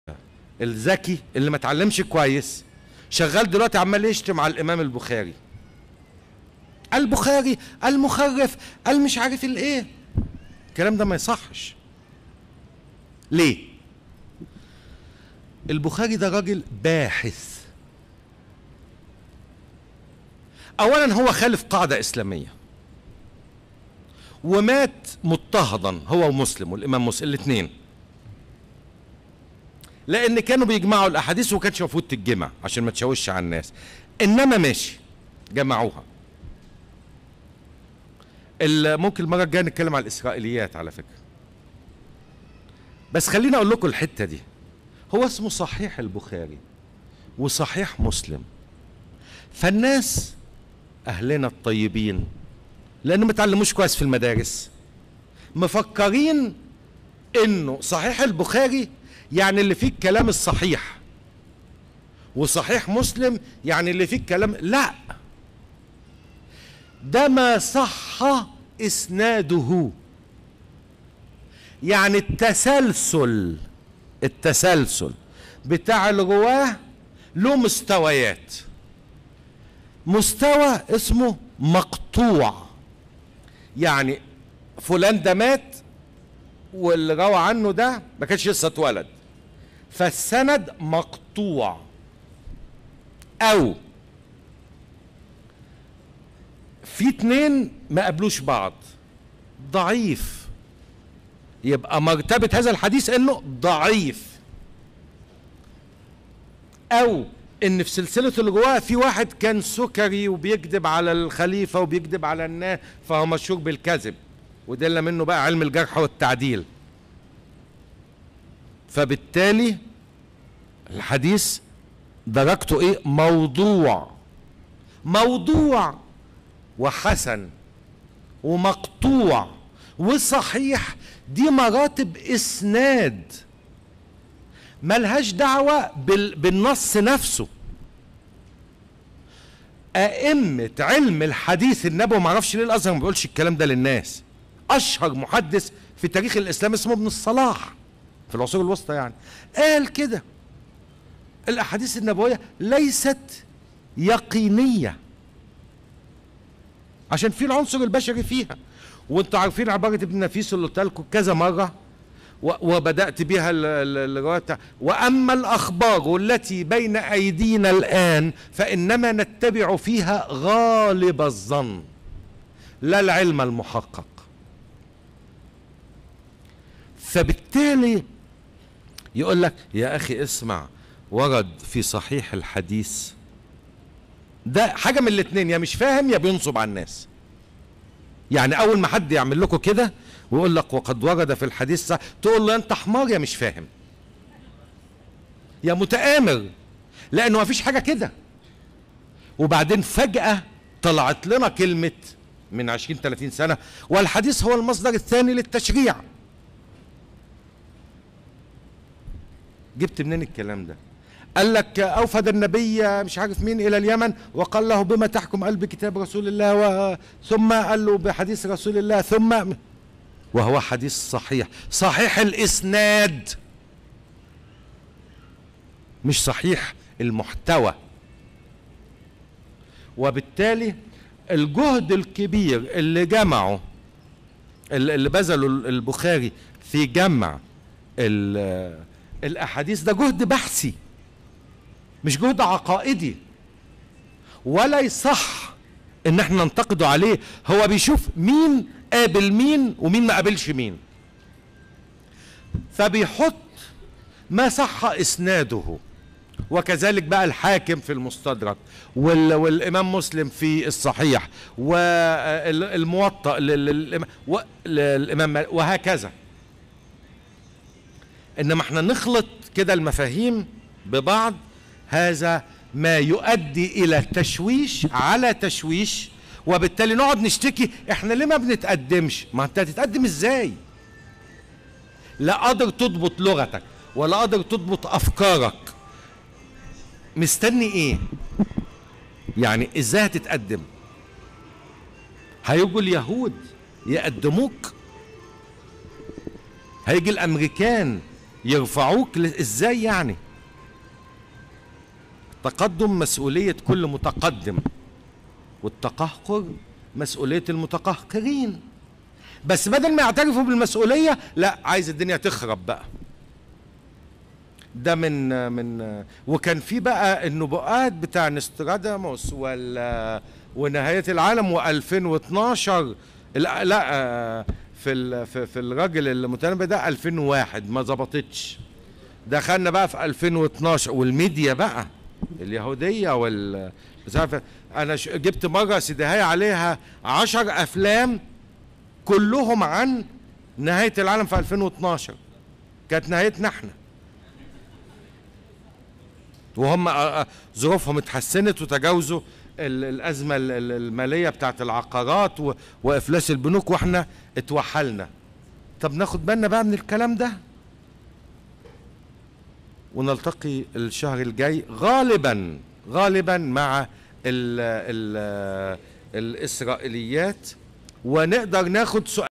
البخارى مات مضطهدا لانه خالف قاعده اسلاميه
الدكتور يوسف زيدان يشرح لماذا مات البخارى مضهدا بعصره